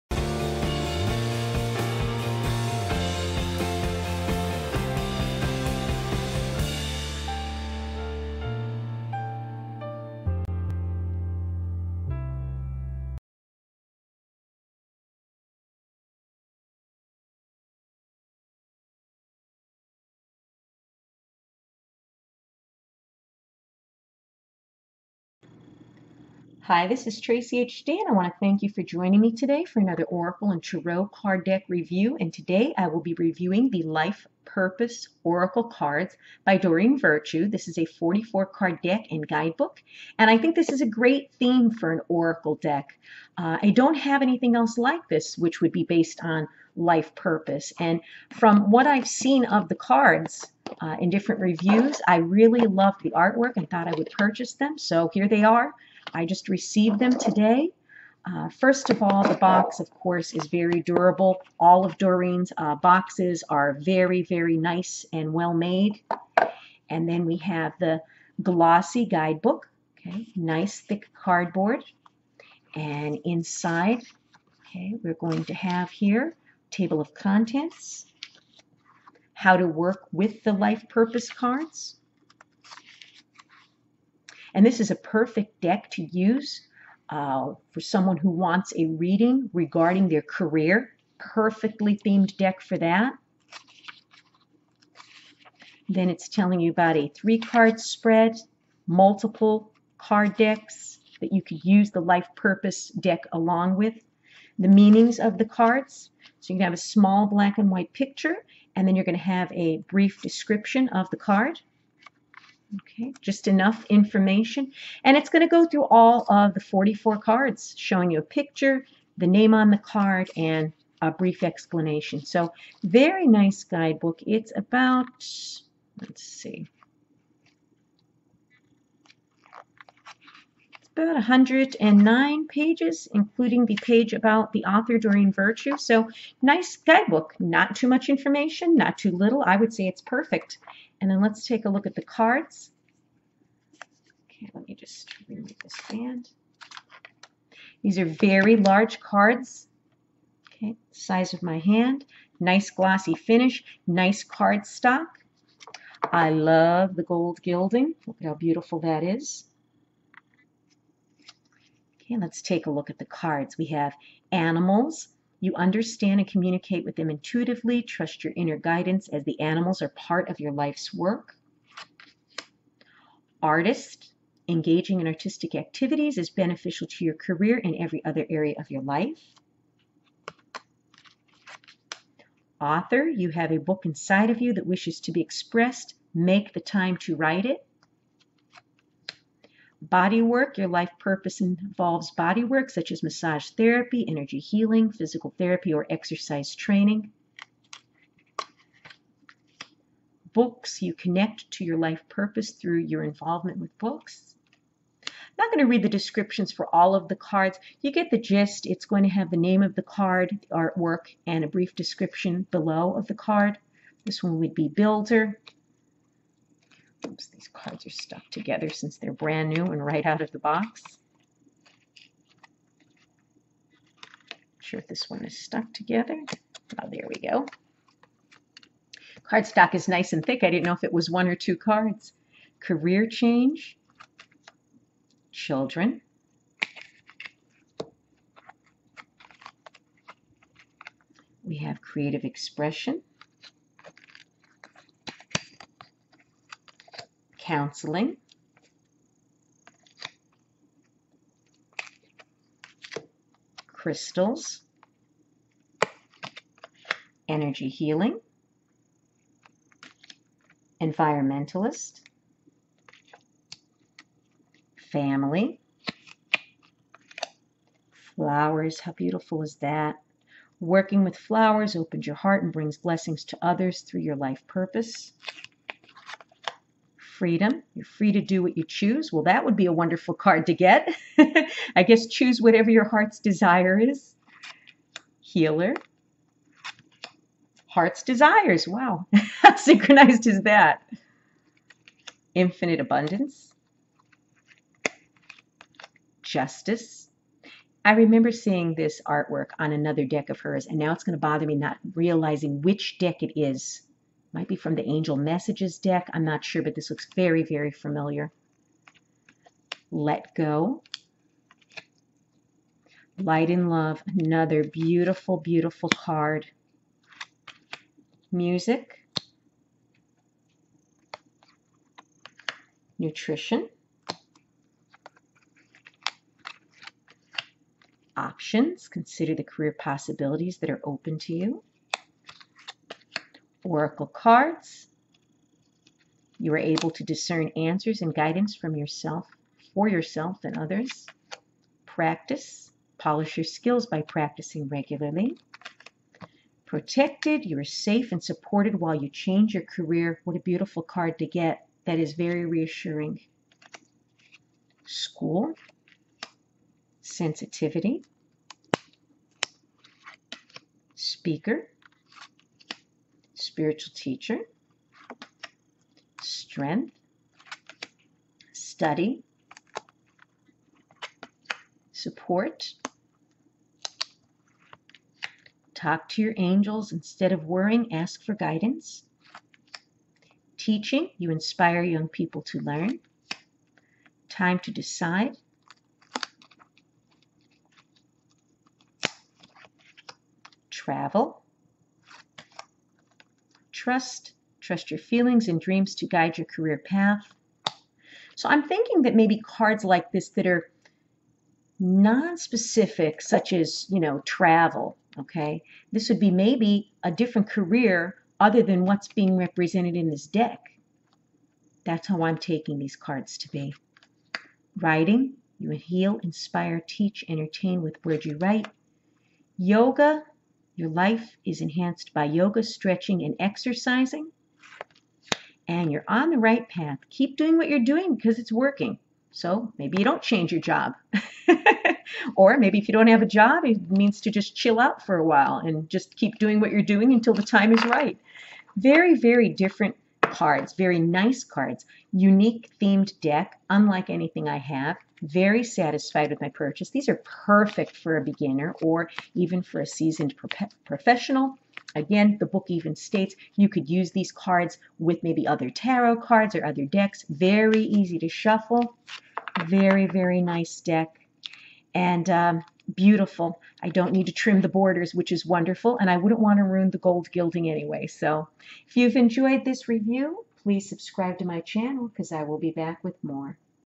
Traceyhd's Review of the Life Purpose Oracle
Traceyhd gives an honest & unbiased review of the Life Purpose Oracle deck by Doreen Virtue. In this very detailed & descriptive review each & every card is shown along with the included guidebook. Don't Miss a review.